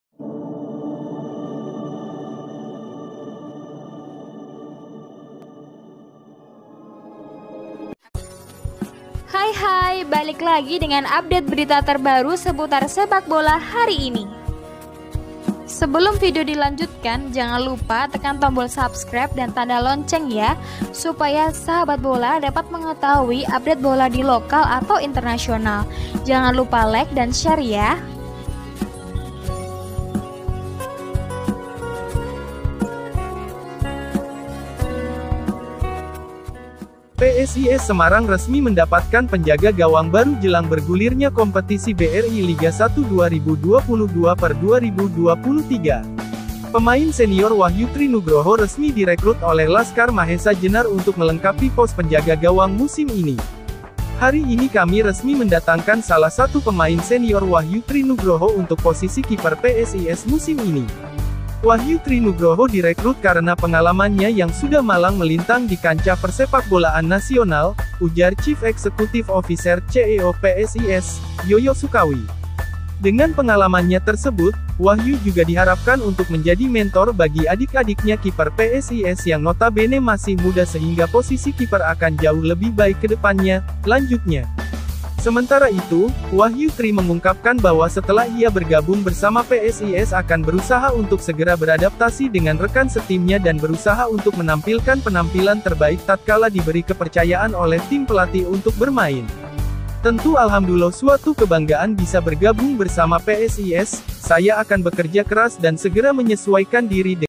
PSIS SEMARANG rekrut kiper Baru
Saat ini Wahyu Tri sudah tiba di Kota Semarang dan telah menjalani serangkaian tes medis.